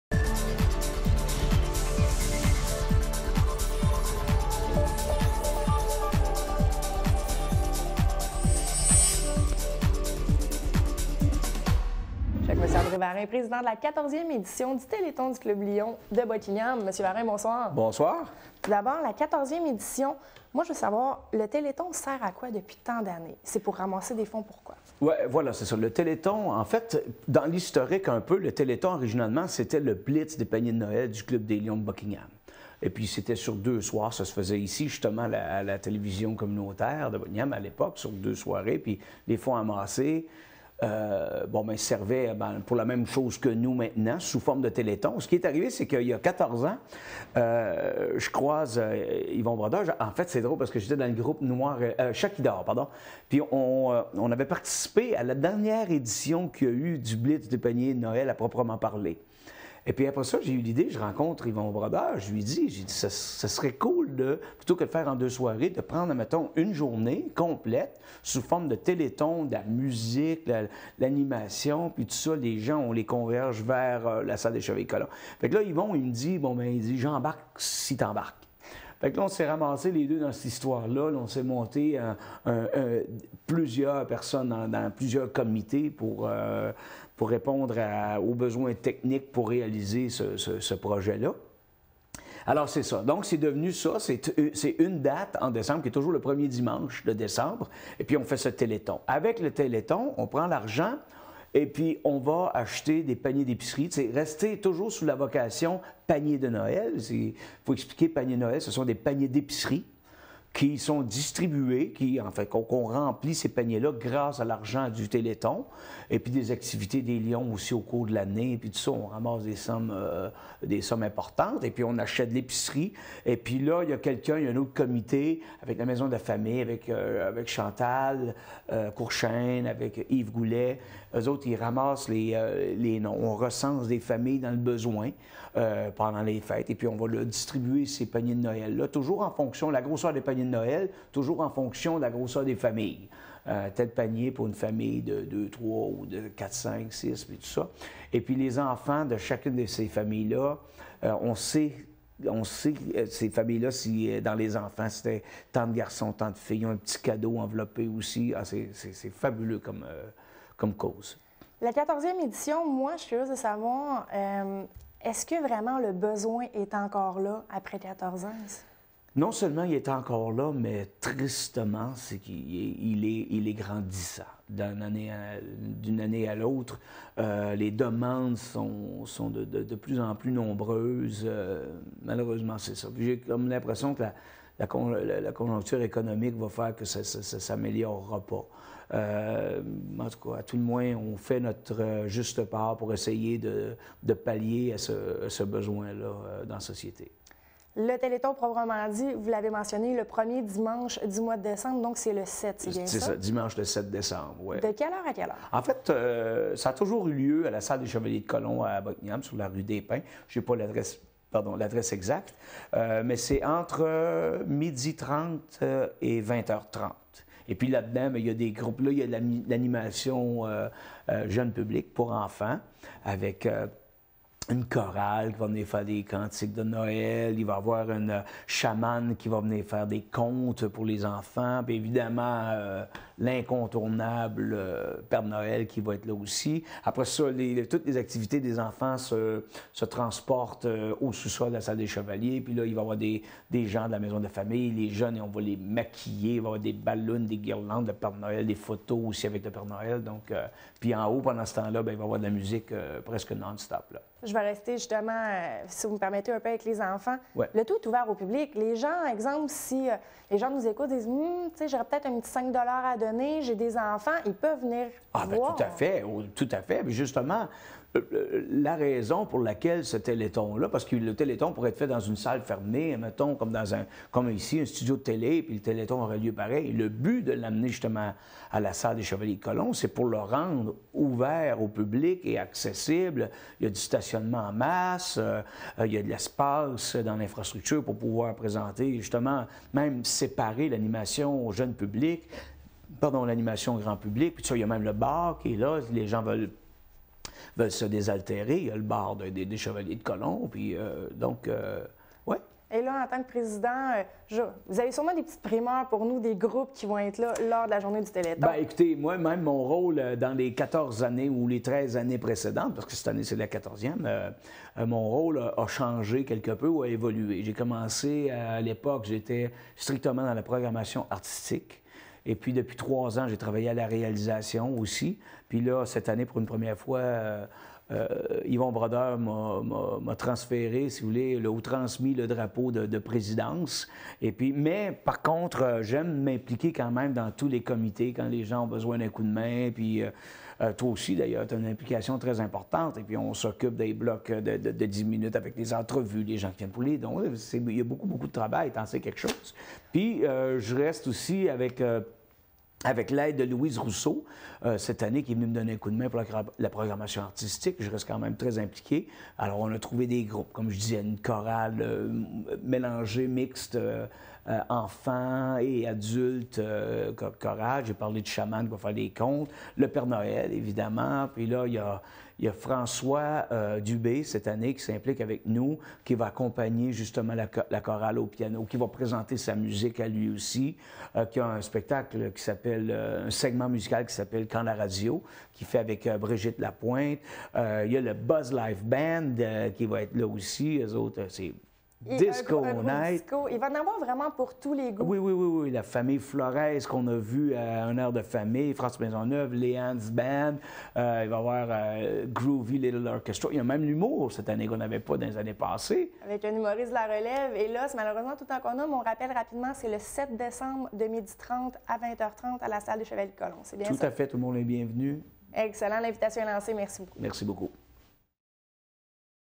Entrevue studio - 27 novembre 2014
Entrevue avec le président du 14e Téléthon du Club Lions de Buckingham, monsieur André Varin.